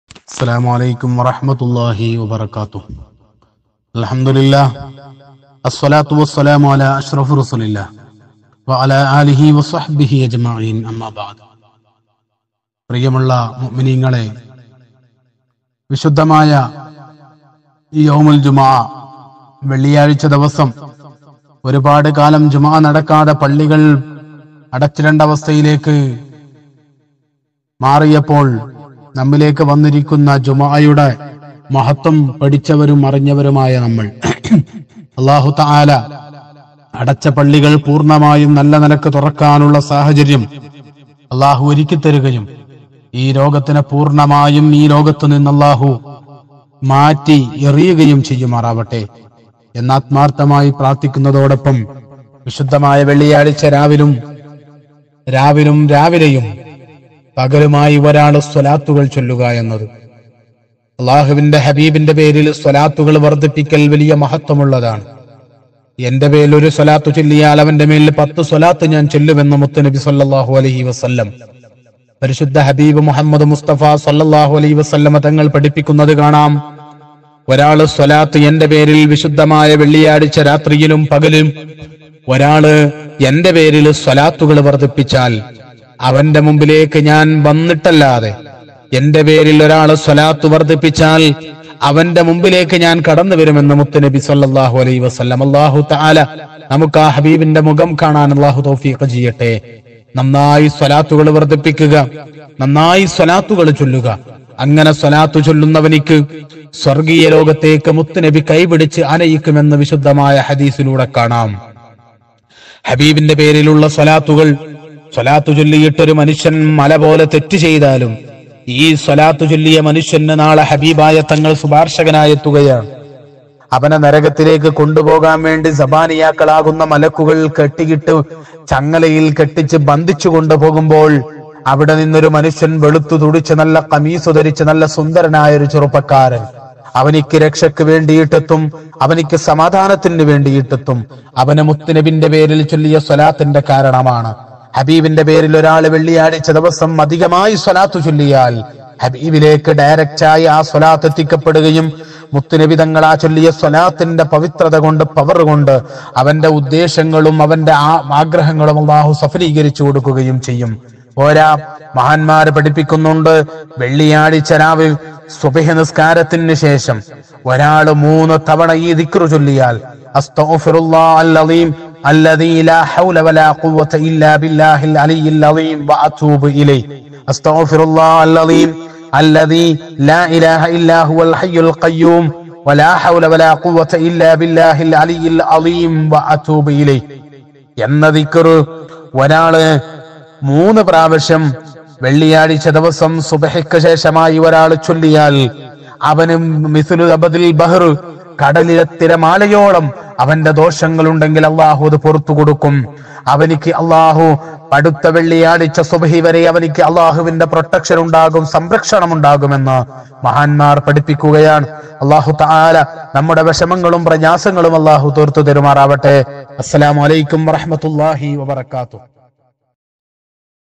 വെള്ളിയാഴ്ച ദിവസം ഒരു വട്ടം സ്വലാത്ത് ചൊല്ലിയാൽ രക്ഷപ്പെടും | Friday Islamic Speech | PowerFul dhikr
Malayalam Islamic Speech | Shafi Media
Hassainar Shahadi Charladka

Subject
വെള്ളിയാഴ്ച ദിവസം ഒരു വട്ടം സ്വലാത്ത് ചൊല്ലിയാൽ രക്ഷപ്പെടും

വള്ളിയായ്ച്ച ദിവസം സുബിഹിക് ഷാശം ഇ ദിക്ർ 3 വട്ടം ചൊല്ലിയാൽ കിട്ടുന്ന നേട്ടങ്ങൾ

വെള്ളിയാഴ്ച ദിവസം ഒരു വട്ടം സ്വലാത്ത് ചൊല്ലിയാൽ ഉണ്ടാകുന്ന അത്ഭുതം

എത്ര തെറ്റ് ചെയ്തവനും രക്ഷപ്പെടും

എല്ലാ പ്രശ്നങ്ങൾ മാറും ഉദ്ദേശങ്ങൾ പൂർത്തിയാവും

സ്വലാത്ത് ചൊല്ലിയാൽ എല്ലാത്തിലും നിനക്ക് വിജയിക്കാൻ കഴിയും

ഉദ്ദേശങ്ങൾ സാധിച്ചു കുറ്റം
പാപങ്ങൾ പൊരുത്ത കിട്ടാൻ

സ്വലാത്തിനും ദിക്‌റിനും പറഞ്ഞാൽ തീരാത്ത മഹത്വങ്ങളാണ് ഉള്ളദ്